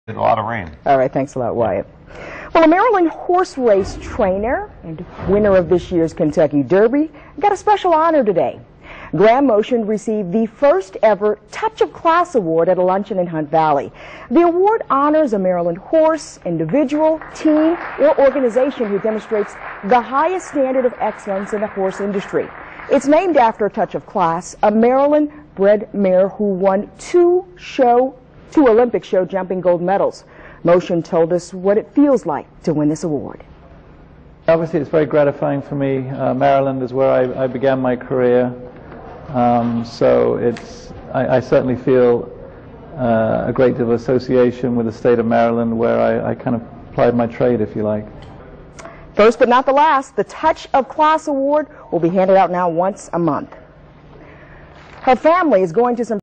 Graham Motion Featured on WMAR-TV for Receiving "Touch of Class" Award
Graham Motion, who trains Sagamore Farm's, Shared Account and won this year's Kentucky Derby with Animal Kingdom, received the Maryland Horse Industry Board's (MHIB) first "Touch of Class" Award. The award was presented following the Board's monthly meeting at a luncheon reception at the Baltimore County Center for Maryland Agriculture in Hunt Valley.